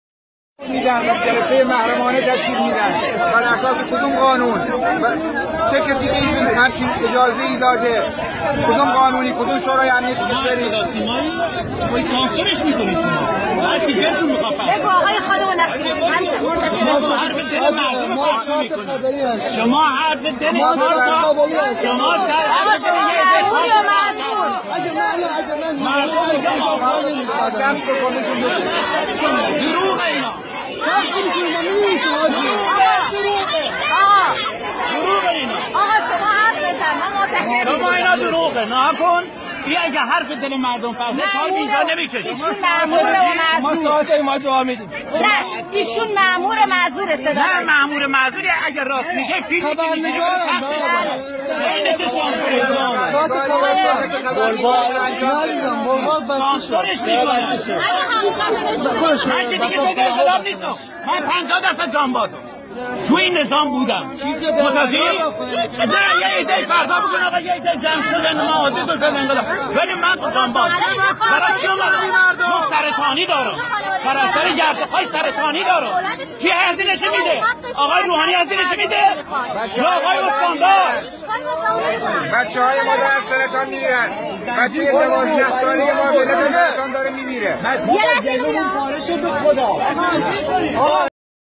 ساکنین خوزستان به جان امده از درد زندگی
ساکنین  خوزستان  به جان امده از درد زندگی در  شرایط اسفبار موجود و از بیماری و سرطان به  سانسور خبری اعتراض می کنند